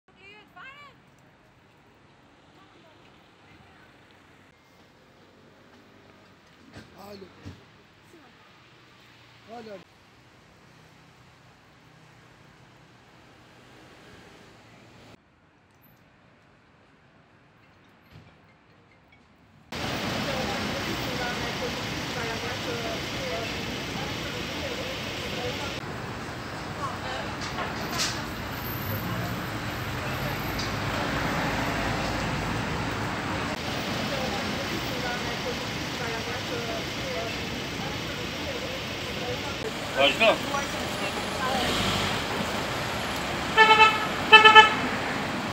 Report TV -Objekt i dyshimtë te 'Rruga e Salës' në Tiranë, përballë ambasadës iraniane!
Një objekt i dyshimtë në tokë është konstatuar në afërsi të rrugës "Mustafa Matohiti", apo e njohur si Rruga e Salës.

Në vendngjarje kanë mbërritur Policia e Tiranës dhe dyshohet për një çantë të harruar.